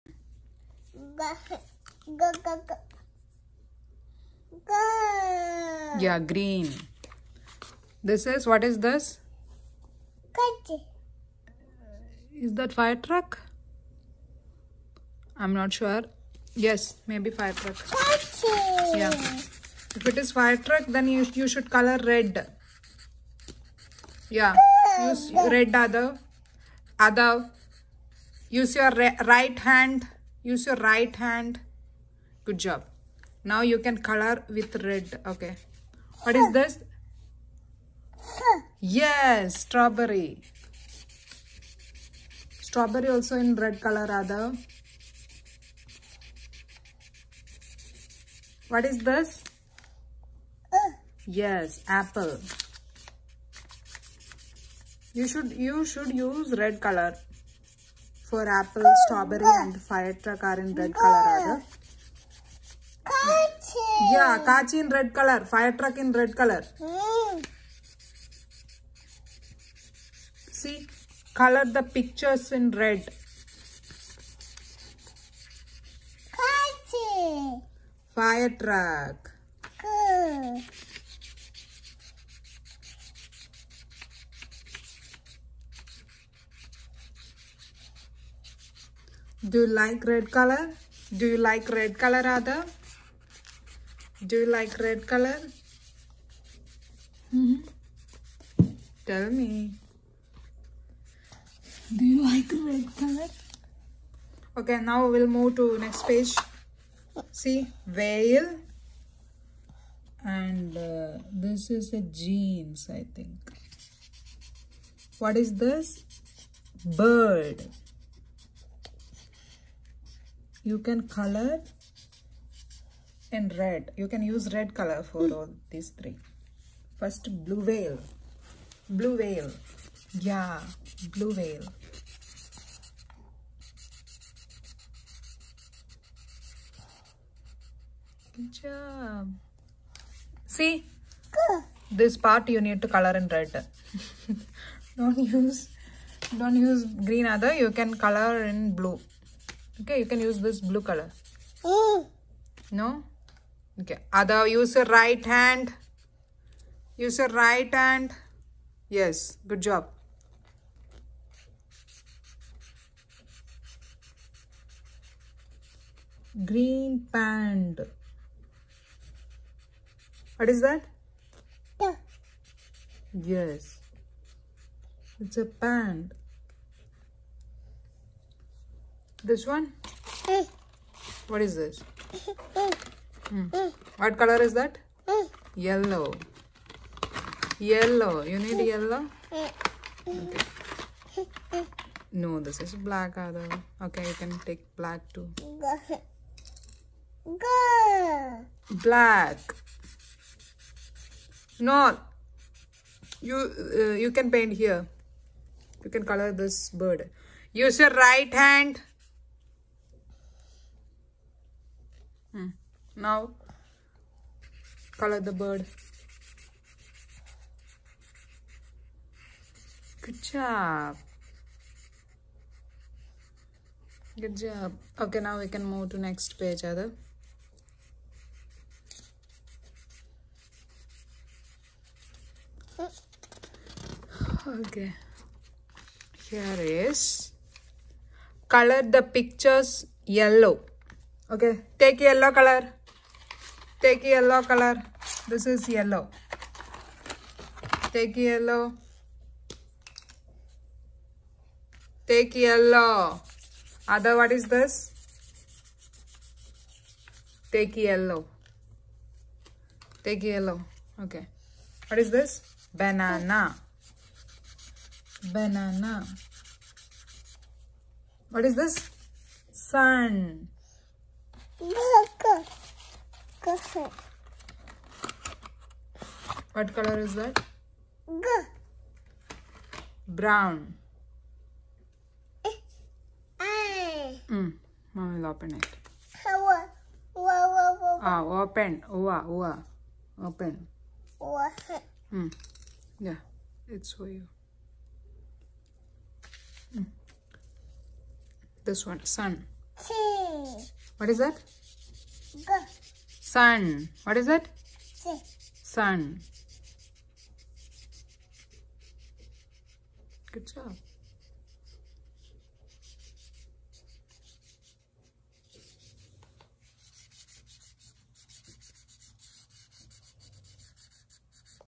Cocomelon coloring Book| How to Learn your toddler to coloring
Cocomelon book coloring, easiest way for f coloring for your toddler